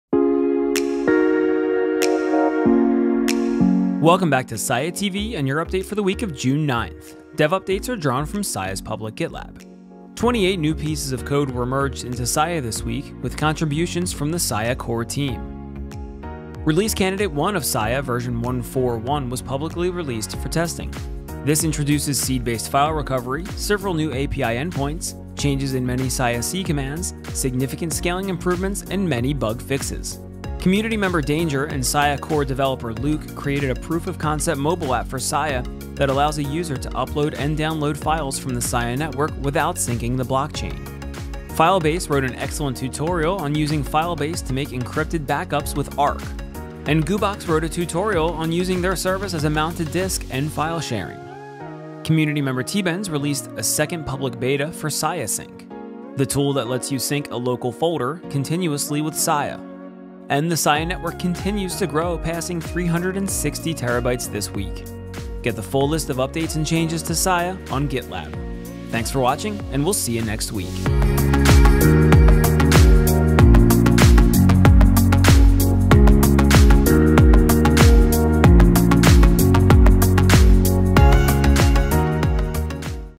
Sia on Mobile First Test - Sia Weekly Update | Week of June 9th
Welcome to the Sia Weekly Update! We'll take you on a quick tour to get you updated on the latest news and info regarding the Sia Storage Platform.